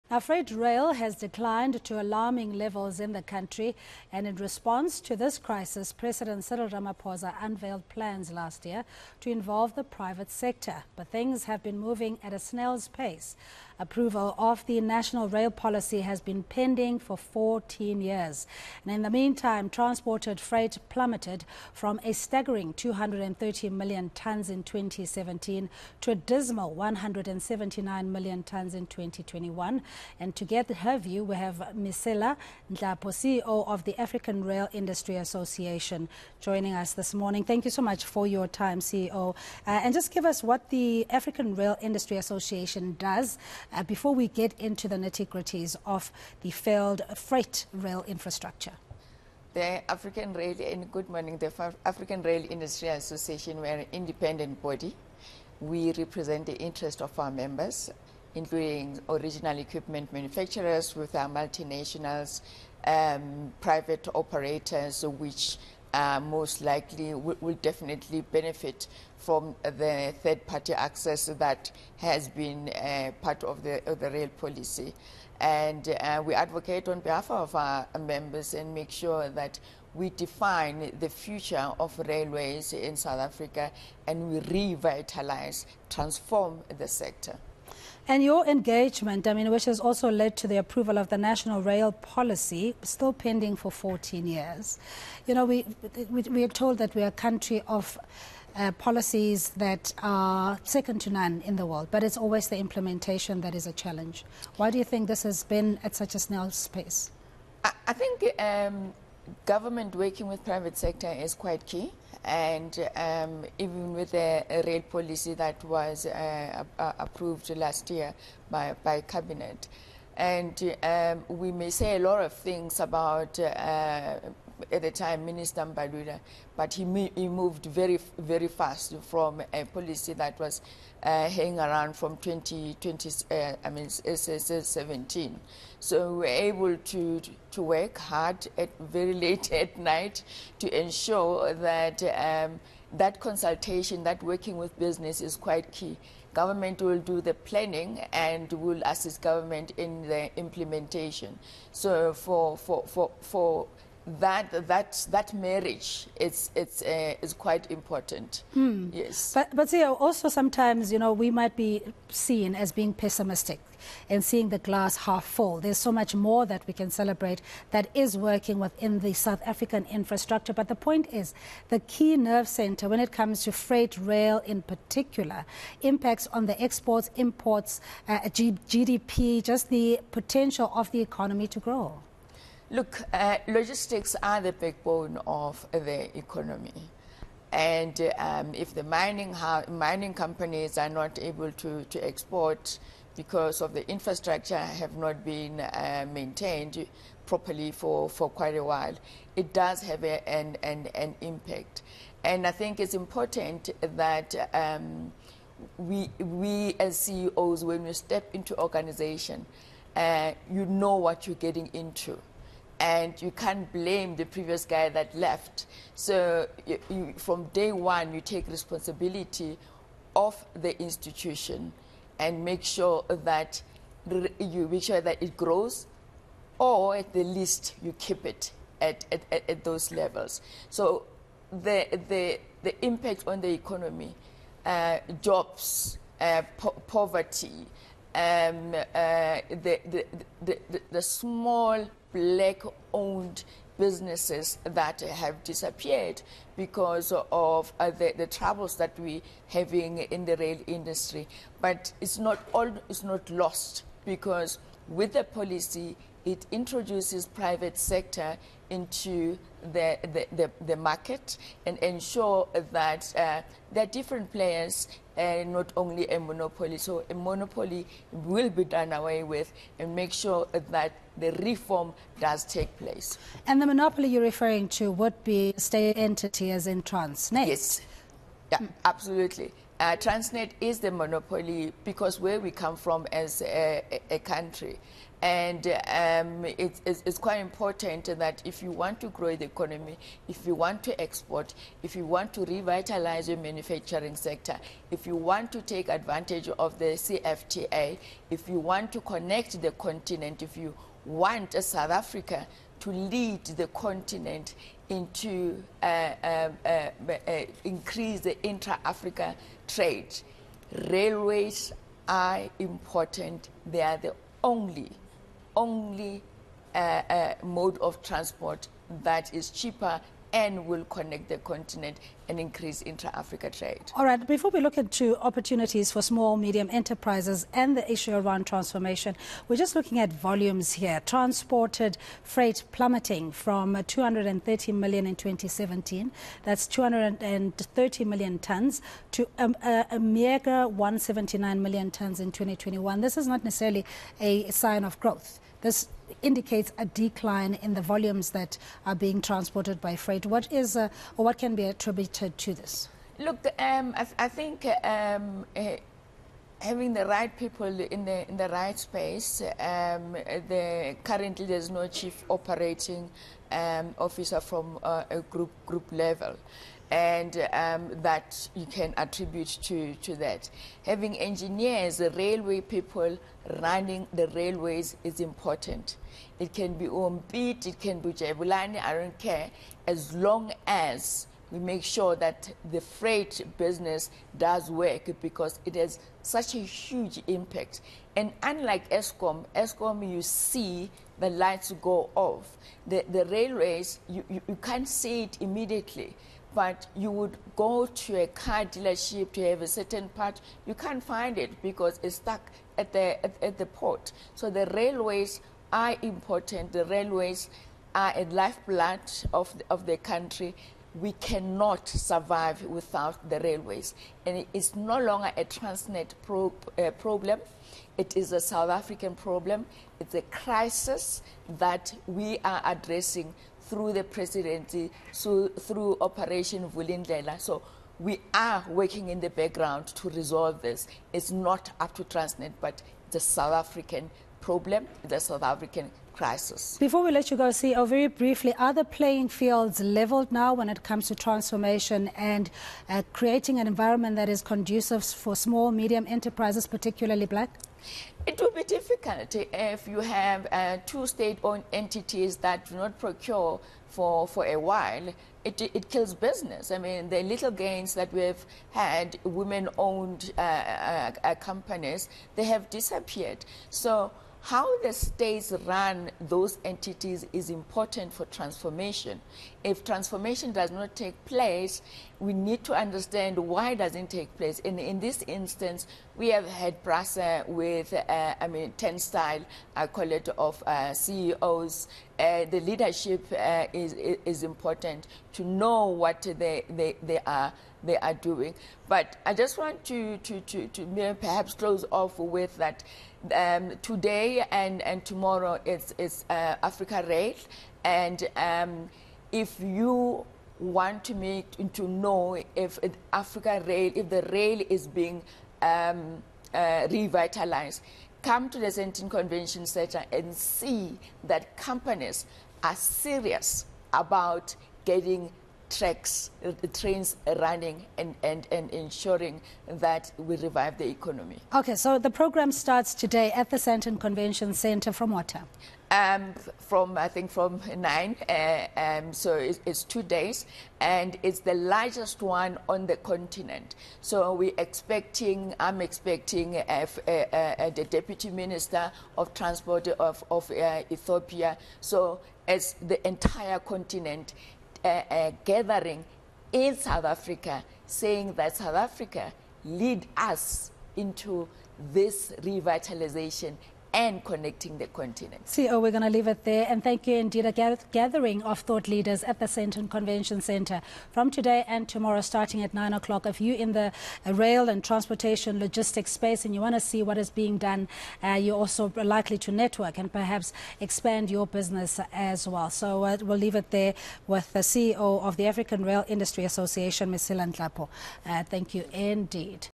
Discussion | Freight rail can boost economy
Freight rail has declined to alarming levels in the country. And in response to this crisis, President Cyril Ramaphosa unveiled plans last year, to involve the private sector. Mesela Nhlapo, CEO of the African Rail Industry Association, spoke to eNCA.